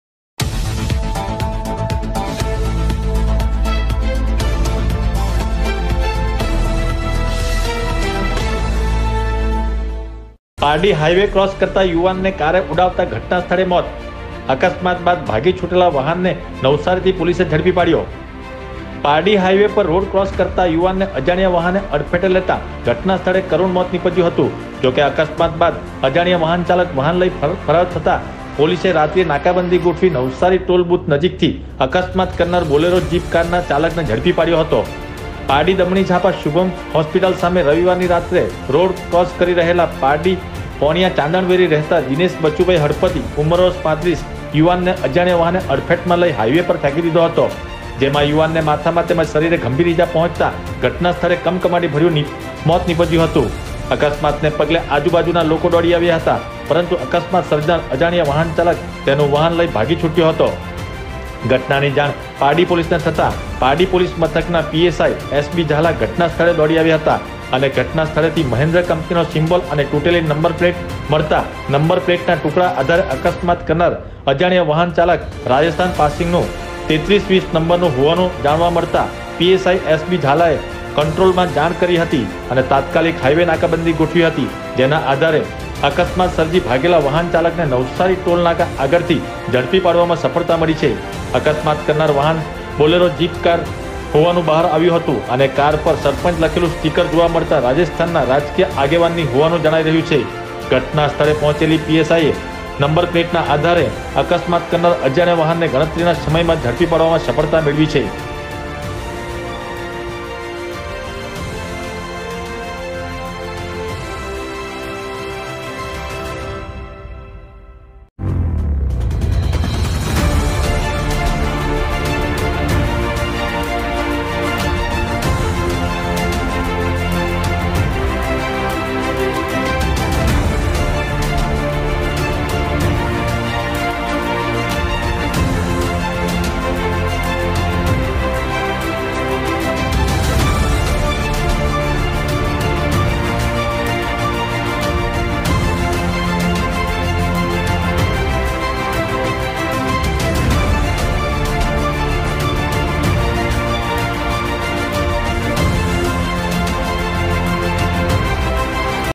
પારડી હાઇવે પર યુવાનને જીપકારે ઉડાવતા નીપજ્યું કરુણ મોત,ભાગેલો કાર ચાલક નવસારીથી ઝડપાયો
અકસ્માત બાદ જીપકાર ચાલક ભાગી છૂટ્યો હતો,
તૂટેલી નંબર પ્લેટના ટુકડા આધારે પોલીસે નાકા બંધી કરી હતી